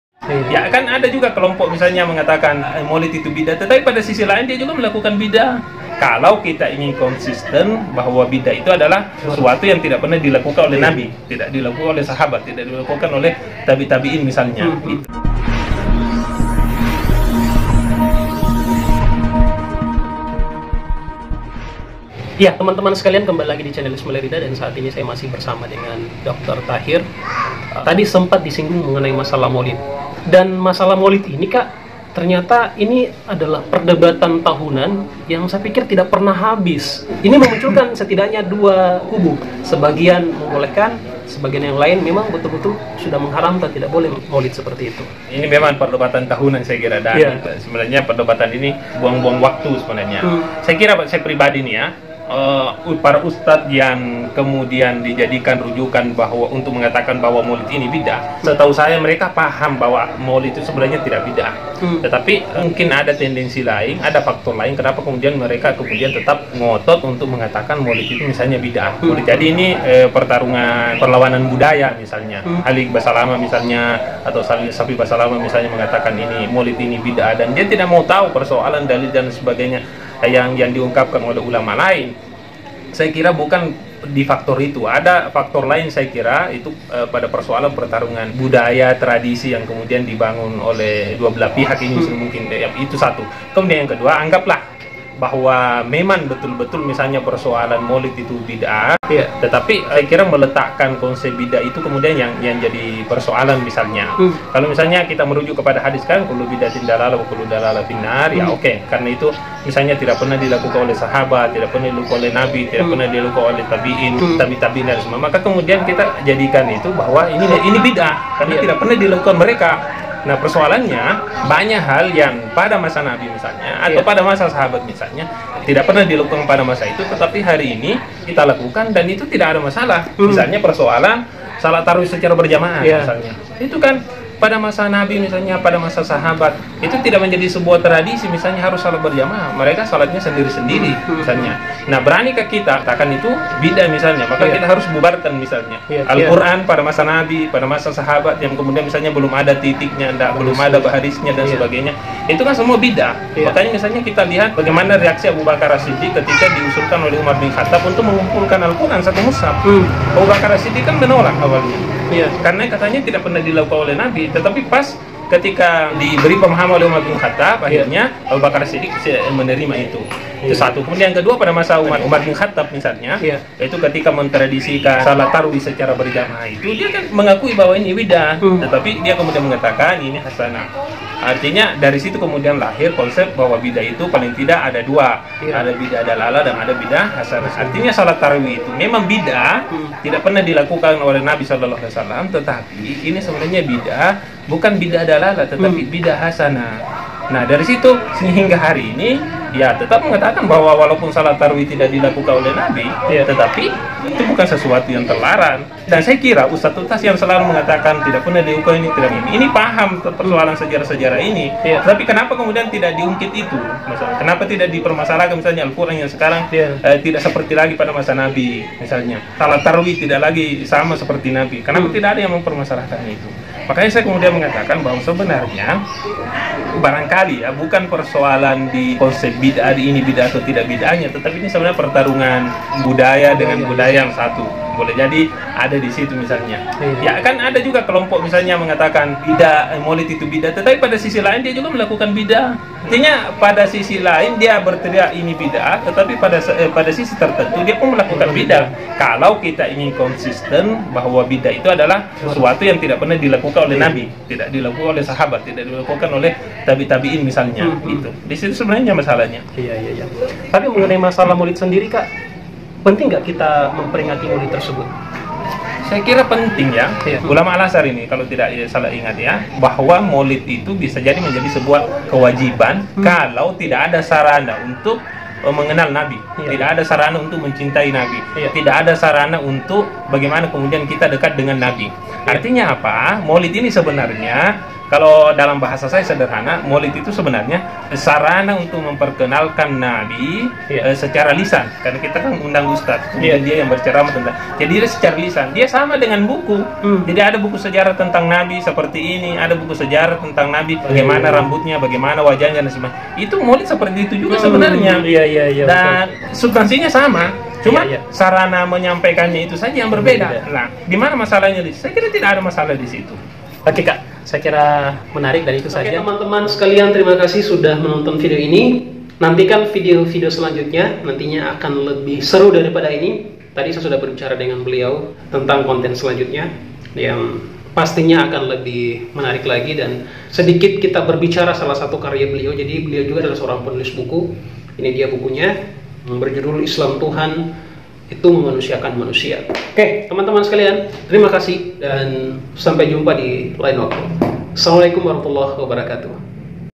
Maulid Bid'ah? Ini Hanya Pertarungan Budaya ‼️ BincangSantai - Ismail Ridha
Setiap tahun, terjadi perdebatan mengenai boleh tidaknya diperingati Maulid Nabi Muhammad Saww. Bagi sebagian orang, Maulid adalah Bid'ah. Tapi bagi yang lainnya, Maulid justru menjadi momen untuk menghadirkan kisah nabi.

Lantas bagaimana seharusnya kita bersikap? Bersama Dr. Muhammad Tahir, kami coba membicarakannya secara singkat di video Maulid Bid'ah? Ini Hanya Pertarungan Budaya ‼️ BincangSantai - Ismail Ridha